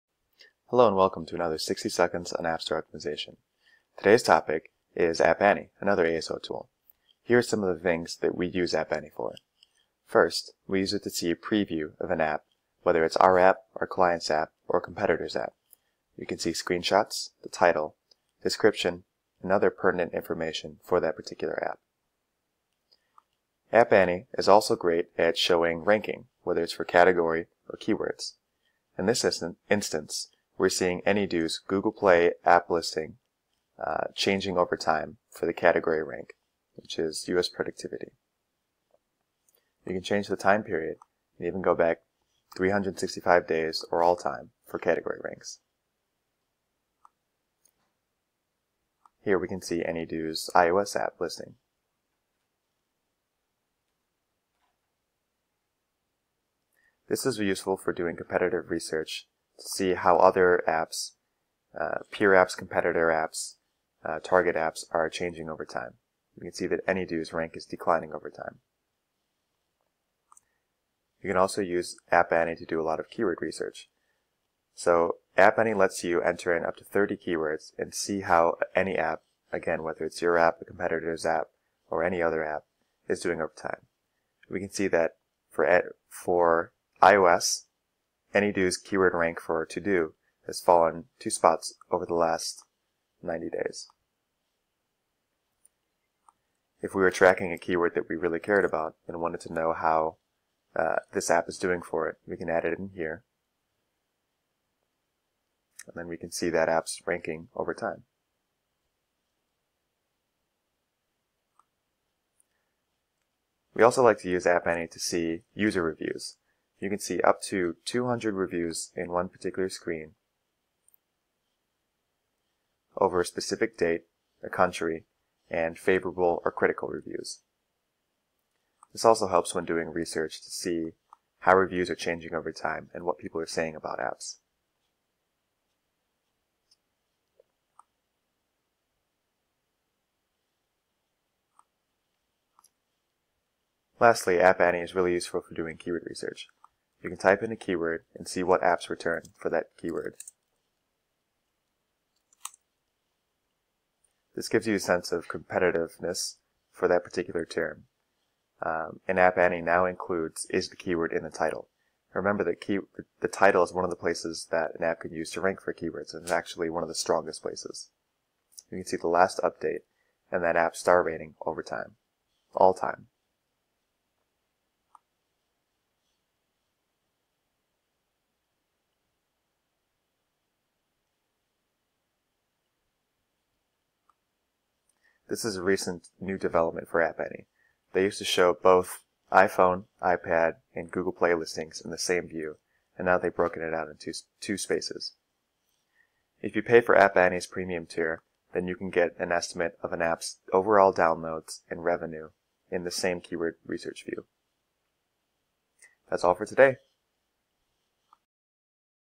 App store optimization: An Overview of App Annie ASO Tool
Get a high-level app store optimization overview of how we use App Annie ASO tool.

Today's topic is ASO tool, App Annie:

We use this to see a preview of apps, whether they be our own, a client's or competitor, such as title, description, and other pertinent information.

App Annie is also great at providing ranking whether it's for category or keywords. You can change the time period for the data displayed. This is how one can view rankings change of time.

You can also use App Annie for keyword research. They allow you to input up to 30 keywords to view ranking for each. If we want to view the ranking for a specific keyword, we can enter it and see the ranking over a period of time.

We also like to use App Annie to see user reviews. You can see up to 200 reviews in one particular screen for a specific country by date.  It's a great way to see what people are saying about apps over time.

Lastly, App Annie is really useful for doing keyword research. You can type in a keyword and see what apps return for that keyword. This gives you a sense of competitiveness for that particular keyword. The latest version of App Annie now tells you if that keyword is in the title of the app.